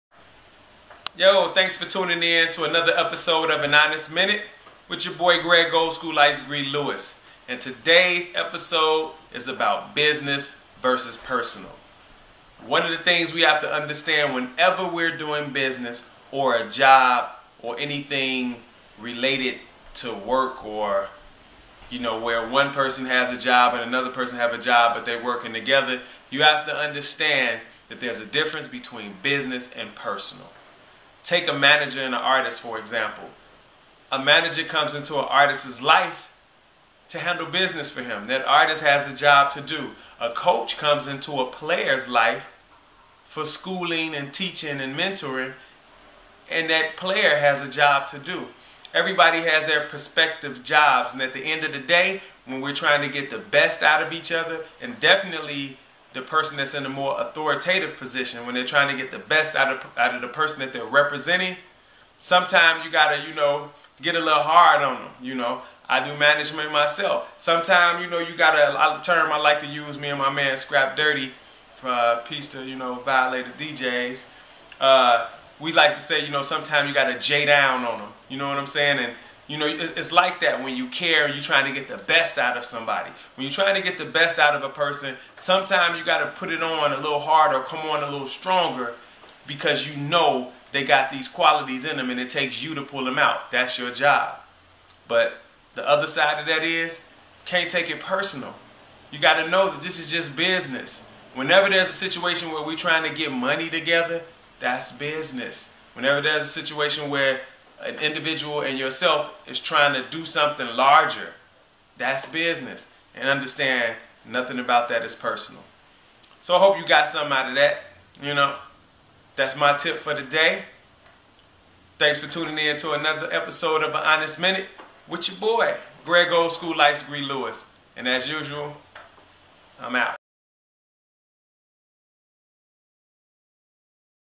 An Honest Minute Episode 19
In this episode of An Honest Minute Greg "Olskool Ice-Gre" Lewis builds on the topic of business -vs- personal.

Twitter Follow: @Olskool_IceGre